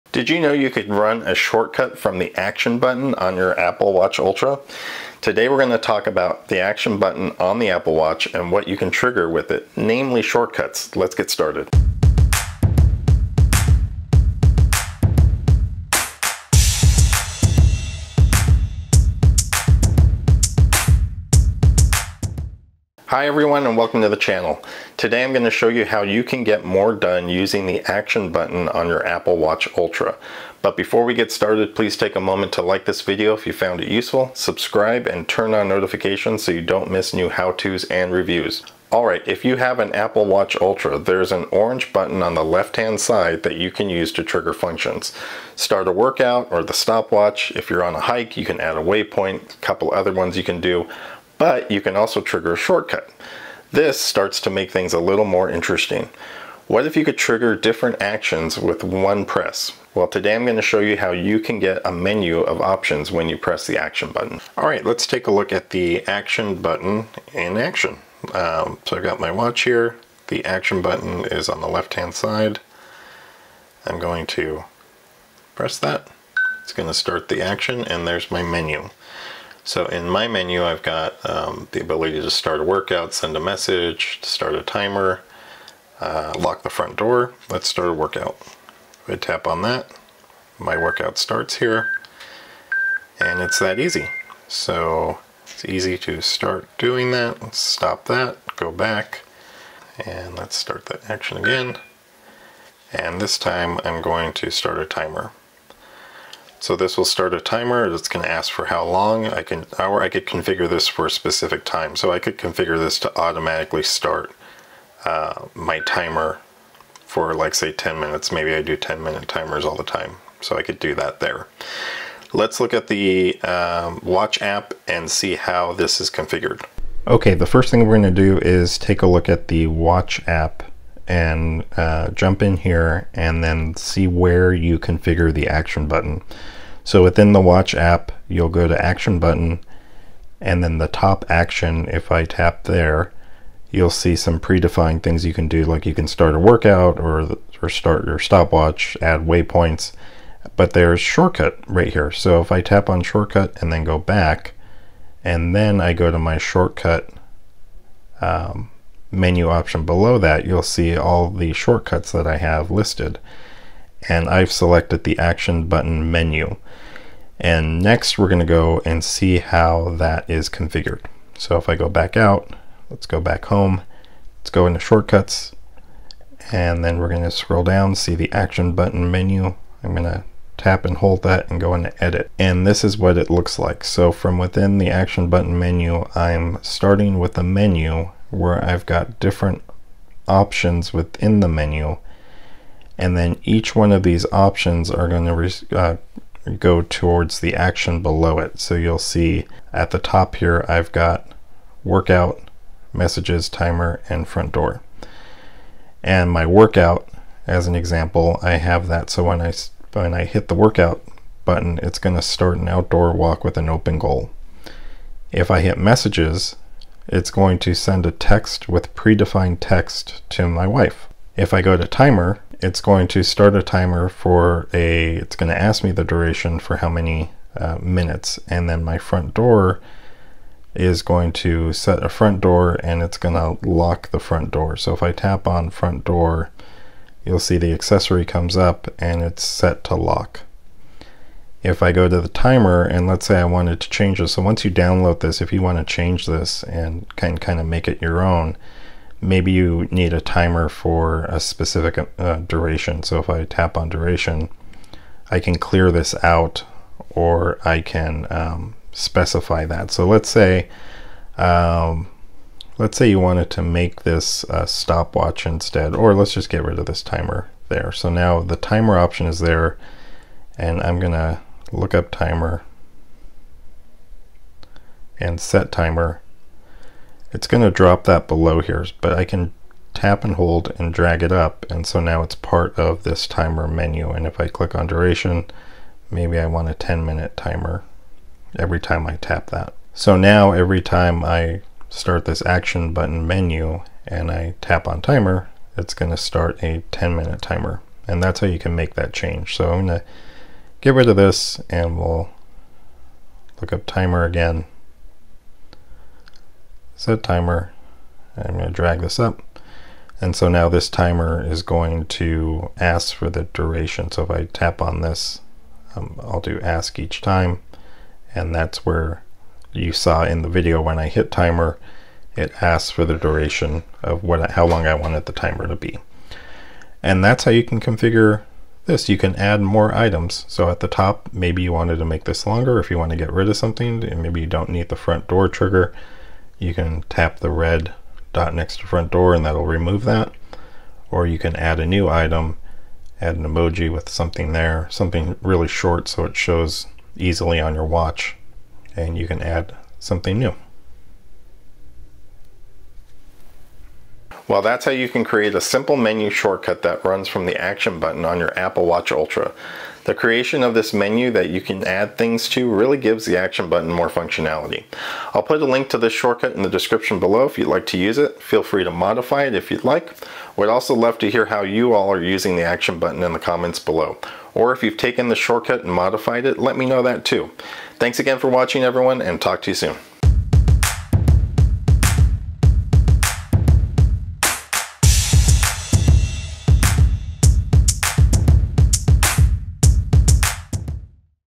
How to Use Shortcuts Apple Watch Ultra!
Did you know you could run a shortcut from the Action Button on your Apple Watch Ultra? Watch this video to find out how to setup a very handy shortcut to take full advantage of the Action Button.

👋🏽 Let’s connect!

⚙️ How I film my content:
📷 Nikon Z30
🎙️ Hollyland Lark Max Wireless Mic

Chapters
00:00 Intro
00:23 Welcome
01:04 Apple Watch Ultra Demo
02:07 How to Setup Shortcut
05:54 Conclusion